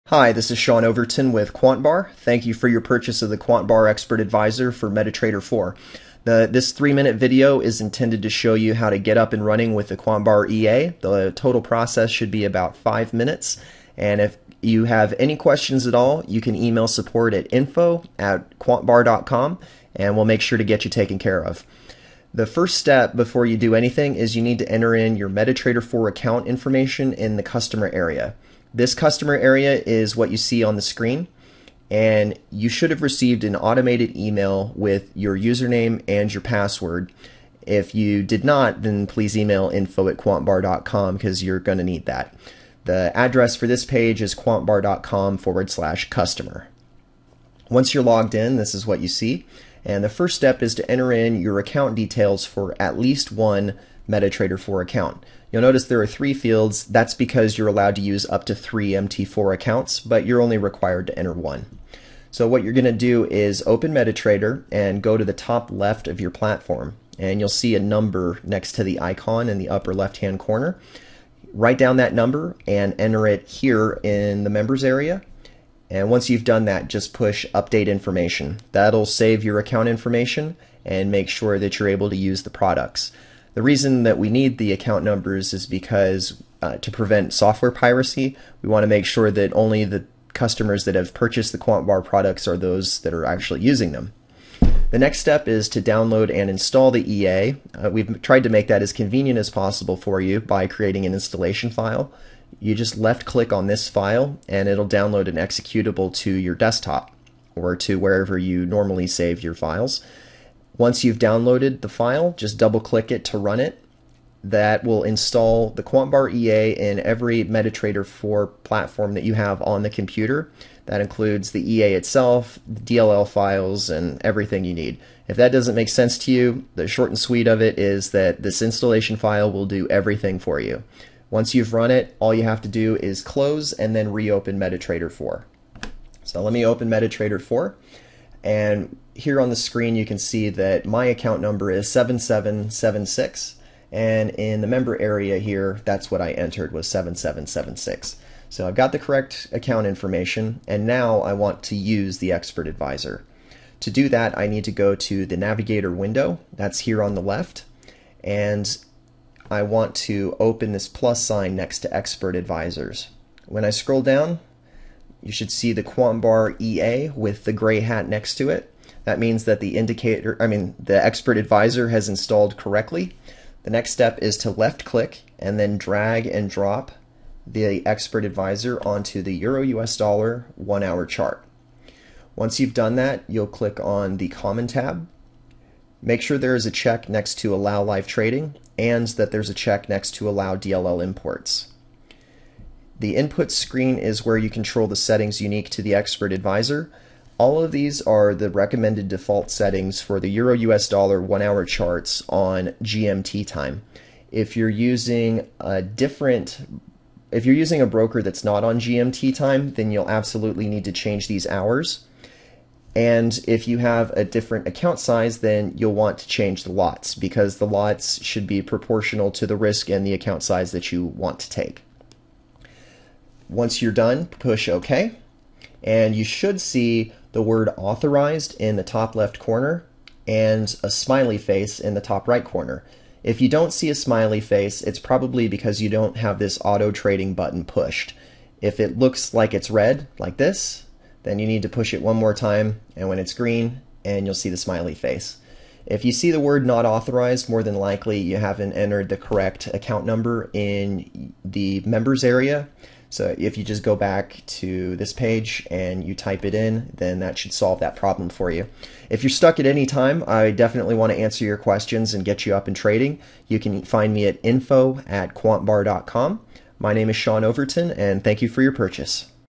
How to install the QuantBar Expert Advisor for MetaTrader 4
A detailed explanation for customers that have purchased the QuantBar expert advisor. The video covers how to use the members area, download the installation file and how to apply the EA to a live, EURUSD H1 chart.